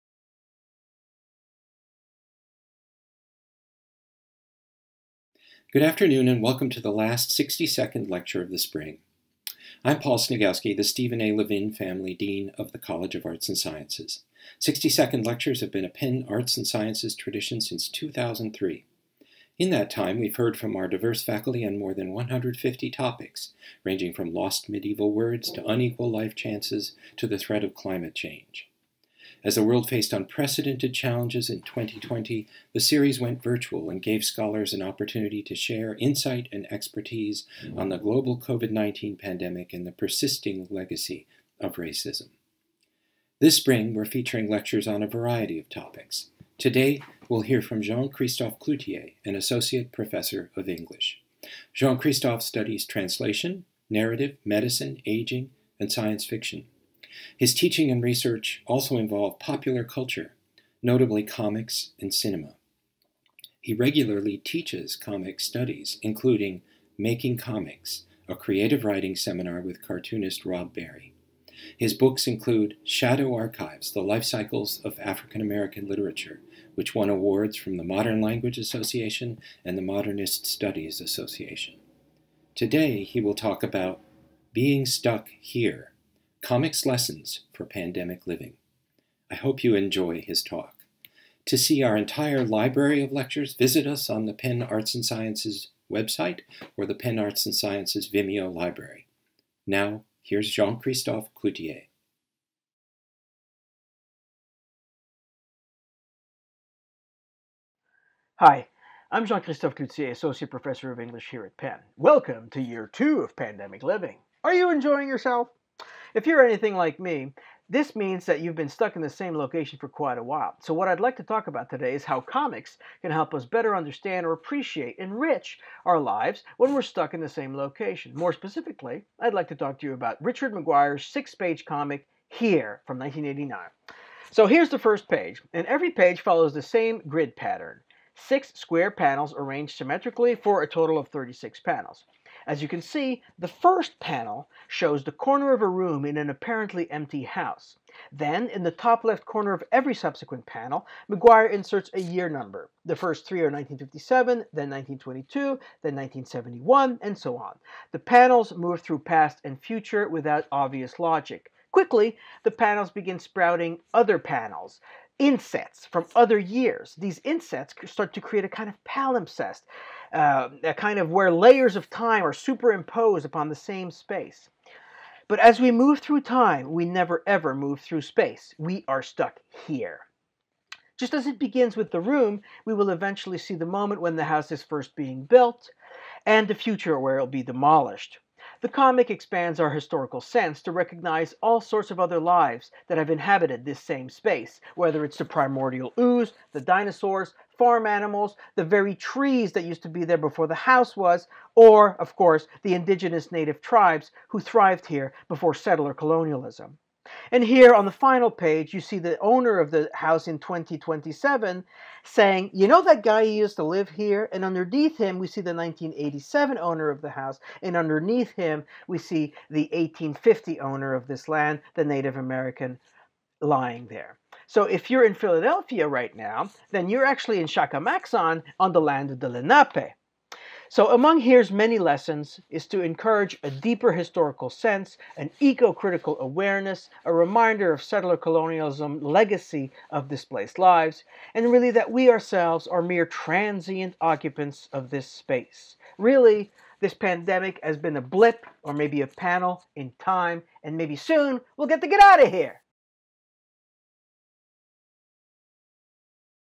60-Second Lectures: "Being Stuck 'Here': Comics' Lessons for Pandemic Living"
Jean-Christophe Cloutier, Associate Professor of English

"Being Stuck 'Here': Comics' Lessons for Pandemic Living" - 1:43